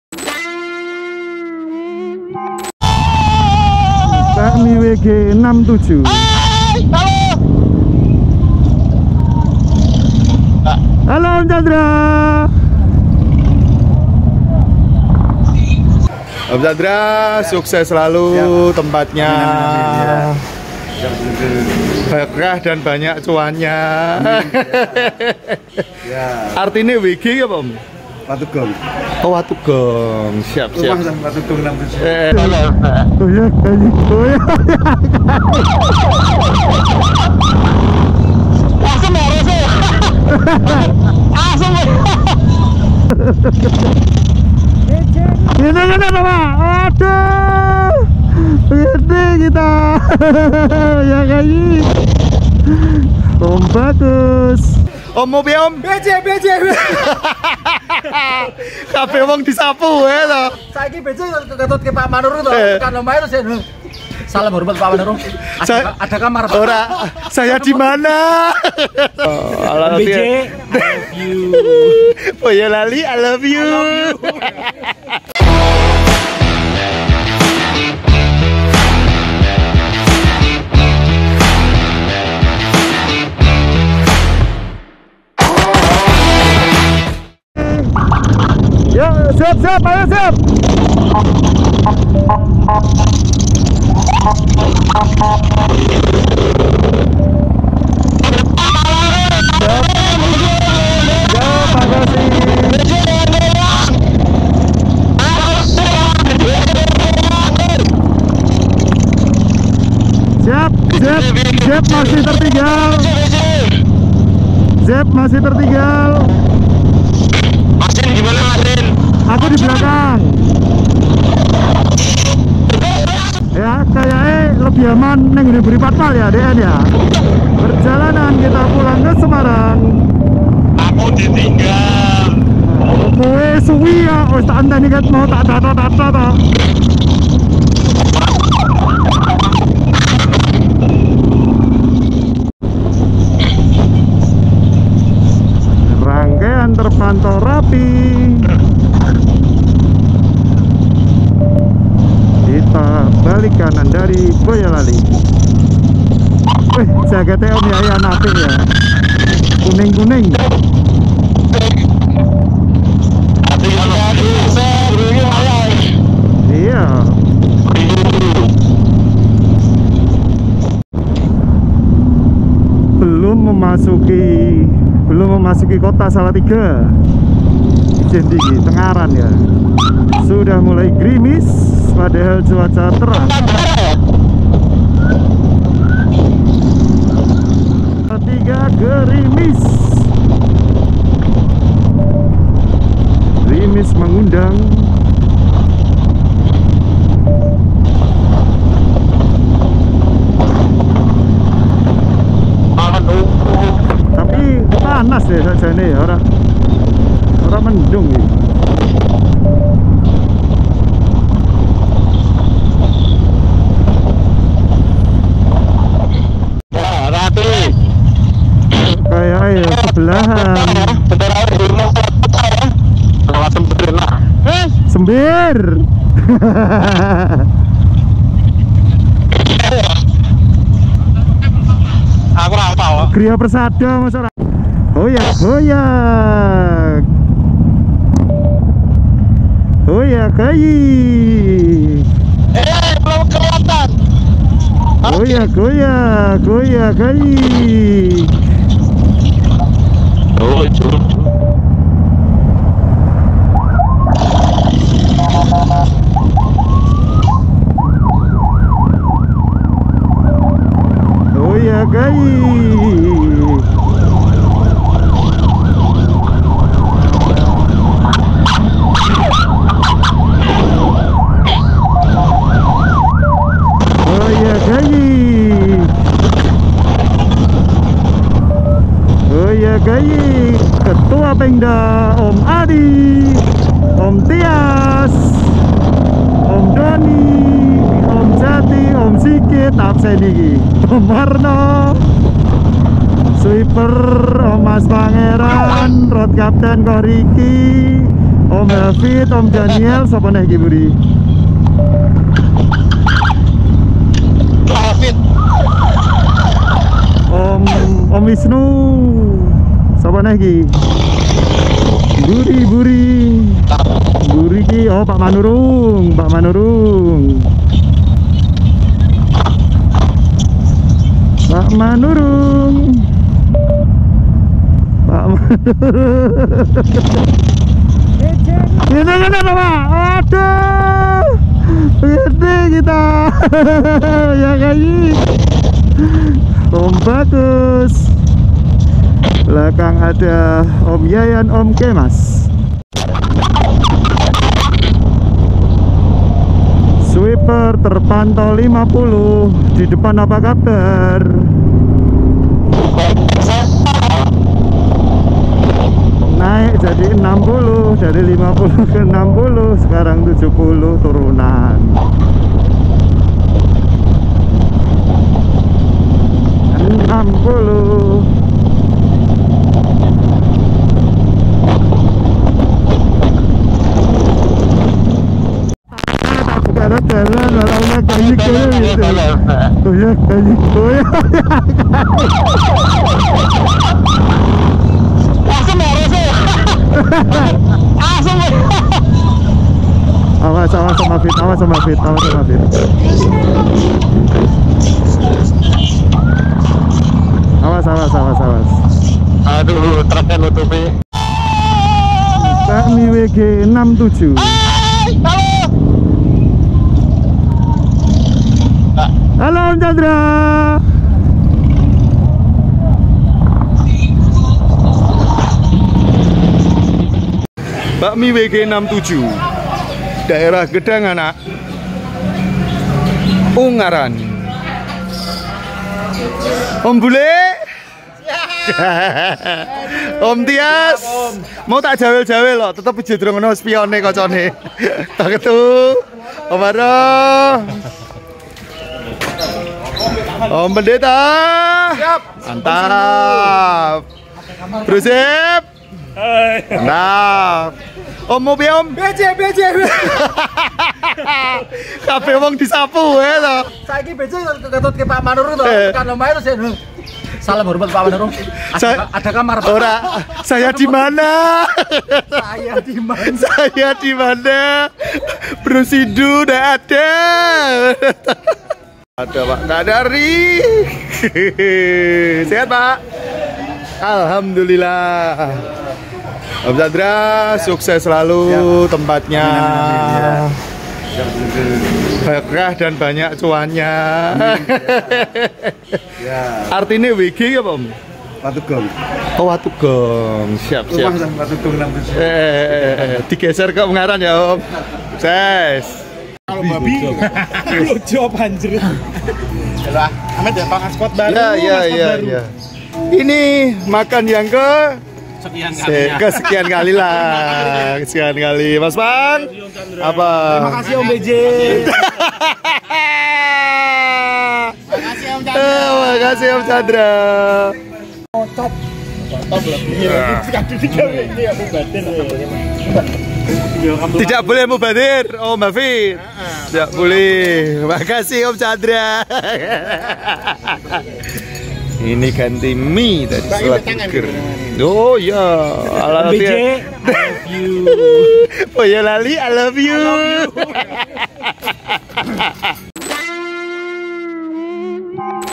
Riding Ke Bakmi WG 67 | Ungaran
Pada hari Sabtu, setelah acara Muscab hdciboyolali selesai
hdcisemarang balik kanan ke Semarang, namun di kabari sama om Chandra, lagi buka bakmi wg 67 di ungaran
dan itu kita lewati, lalu kami beberapa mampir ke situ
untuk harga bakmi sekitar 10rb an
burger nya 15rb an
wenakkkkkkkk

HDCI Hebat
Riding Gak Riding Kumpul